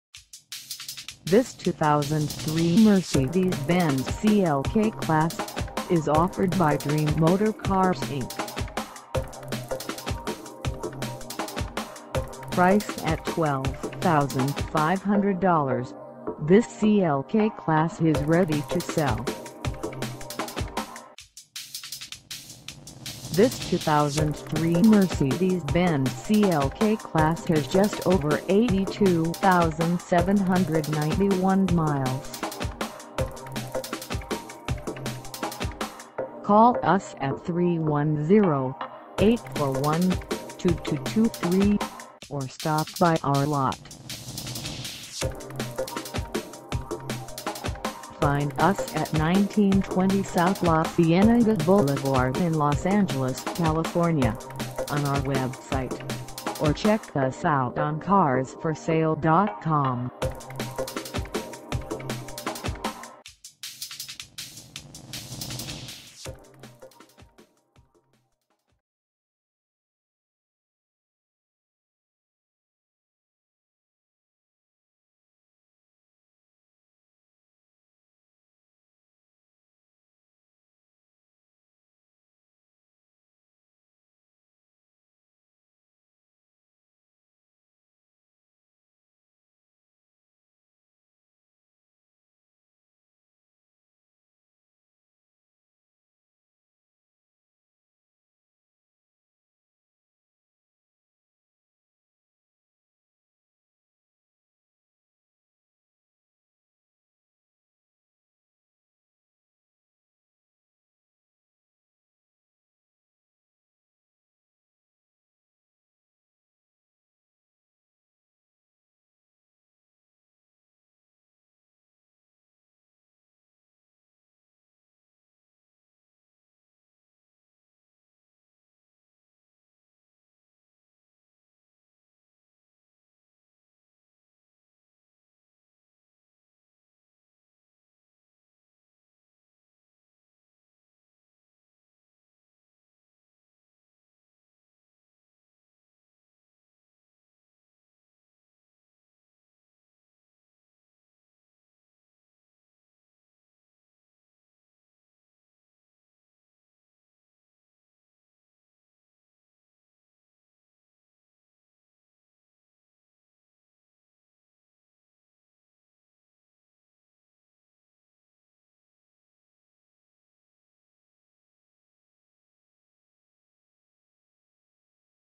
2003 Mercedes-Benz CLK-Class CLK320 Convertible - for sale in LOS ANGELES, CA 90034
DREAM MOTOR CARS INC
1920 S LA CIENEGA BLVD

Come test dirve this 2003 Mercedes-Benz CLK-Class CLK320 Convertible for sale in LOS ANGELES, CA.

Premium Black On Black Leather With Hand Polished Wood Trim .